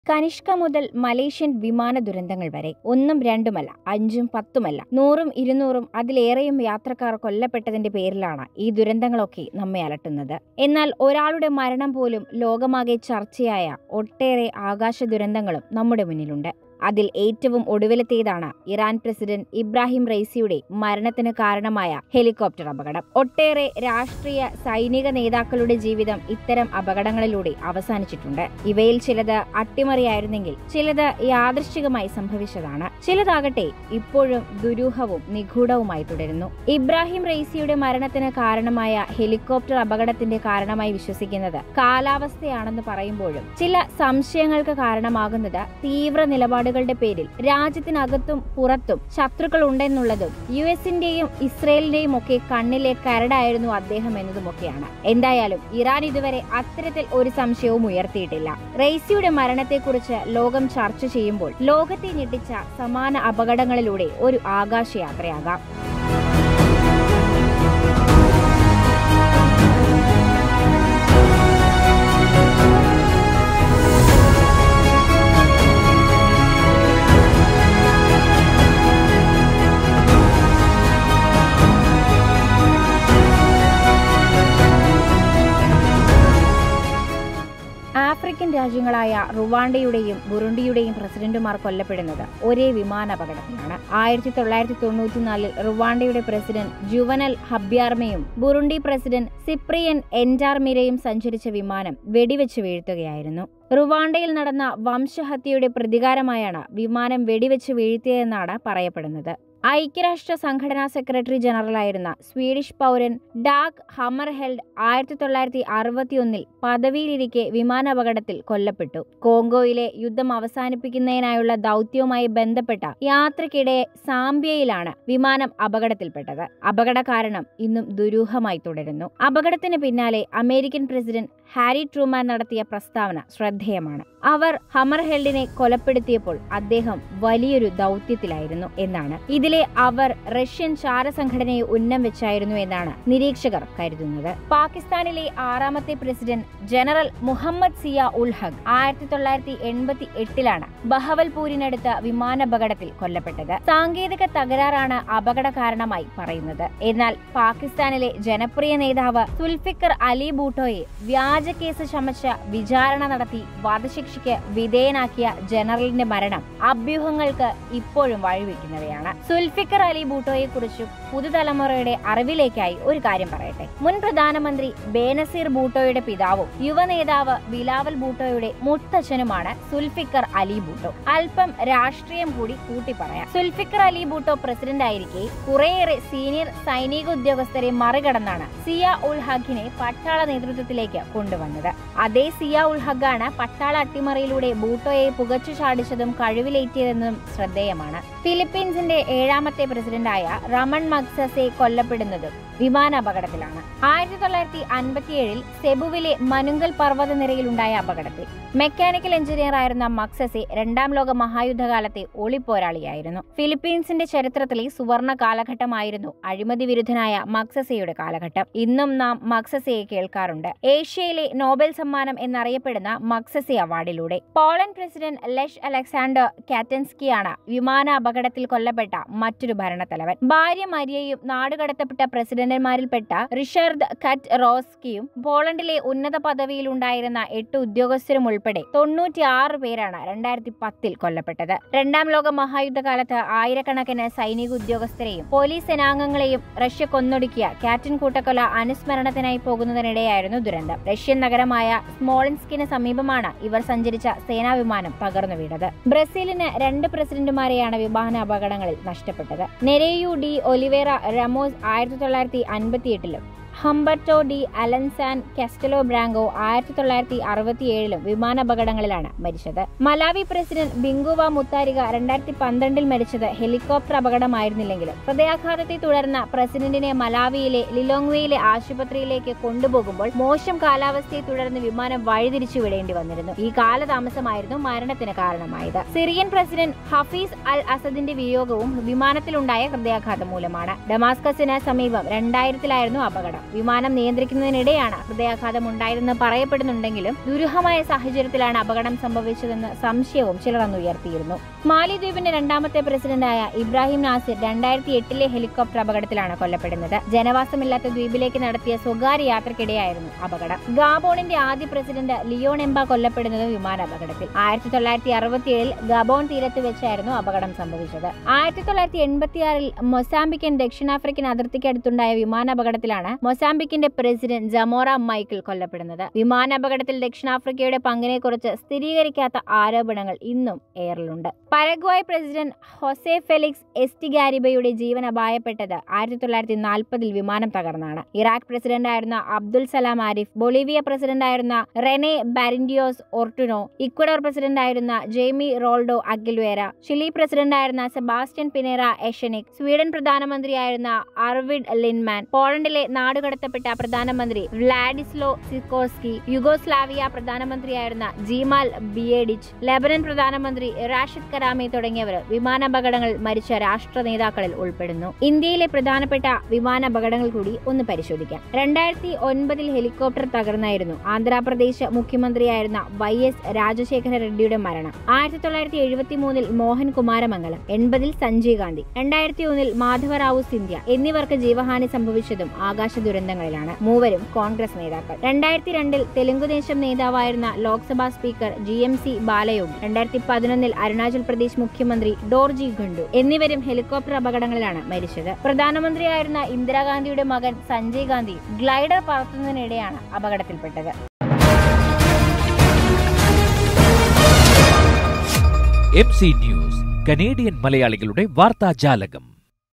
ലോകത്തെ ഞെട്ടിച്ച വിമാനാപകടങ്ങൾ! | Helicopter Crash | Aeroplane Crash | Air Accidents
കനിഷ്ക മുതൽ മലേഷ്യൻ വിമാനദുരന്തങ്ങൾ വരെ…